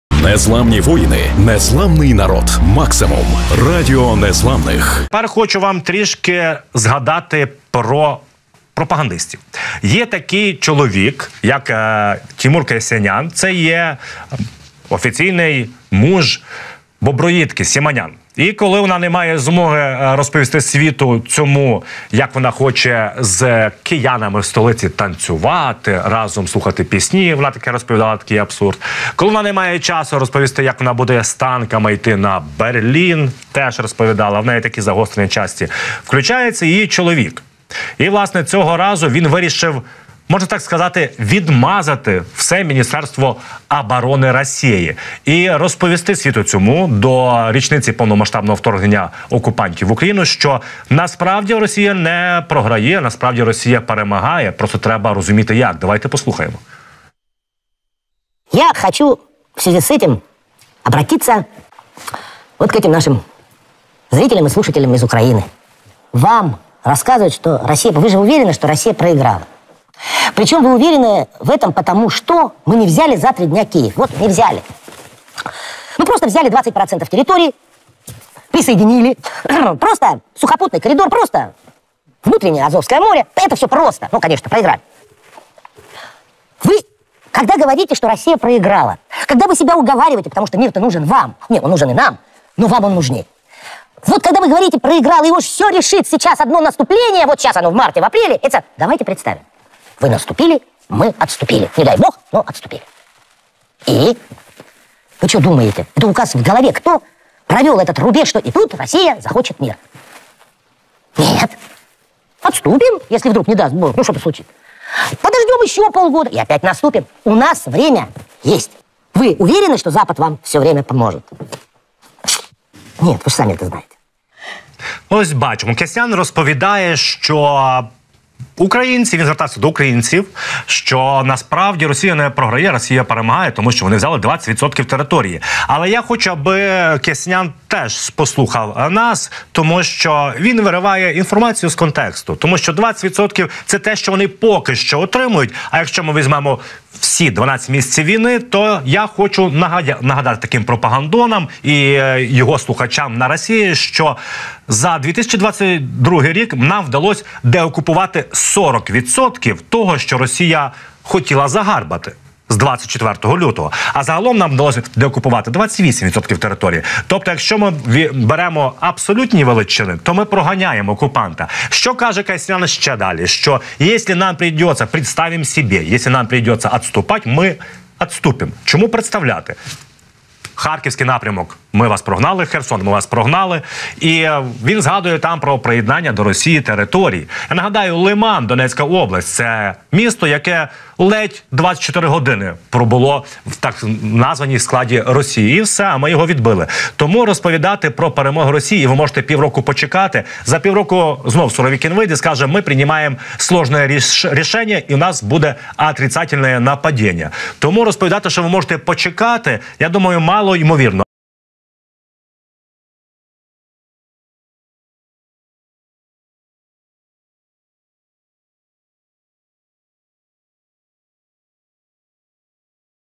"Россия не проигрывает" / Чоловік боброєдки "відмазує" МІНОБОРОНИ РФ
Повномасштабне вторгнення Росії в Україну триває уже рік. Ворожа пропаганда працює не лише на внутрішню авдиторію, але й на зовнішню. Ворог намагається зробити все для того, аби зменшити підтримку України з боку міжнародної спільноти.